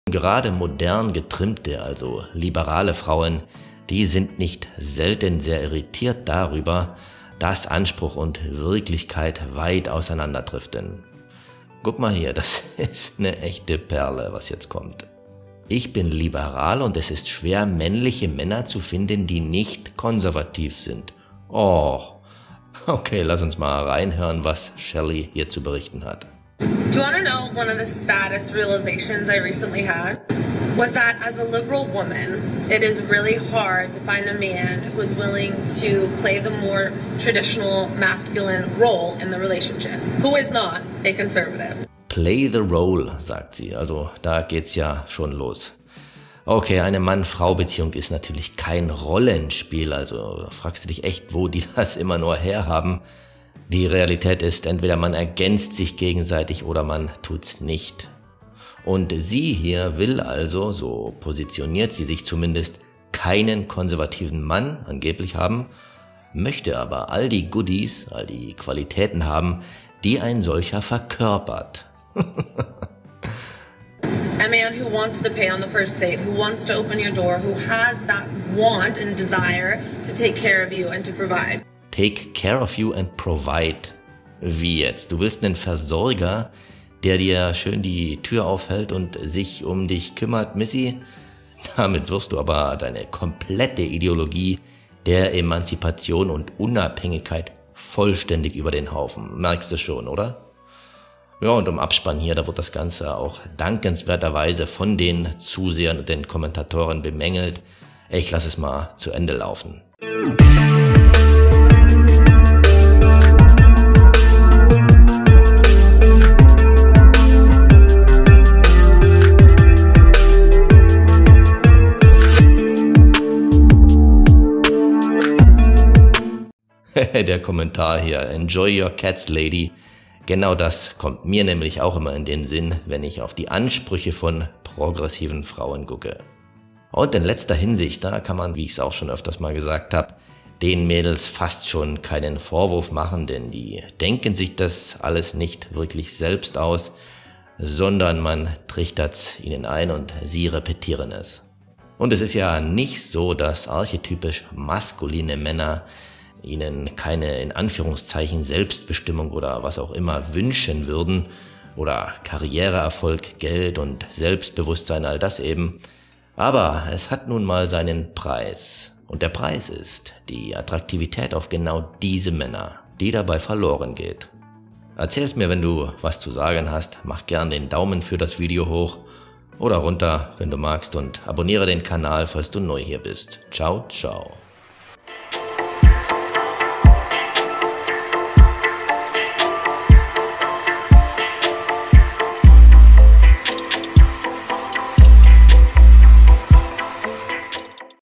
Moderne FRAUEN finden keine GUTEN Männer!
Warum wohl wollen Qualitätsmänner nichts mit modernen liberalen Frauen zu tun haben?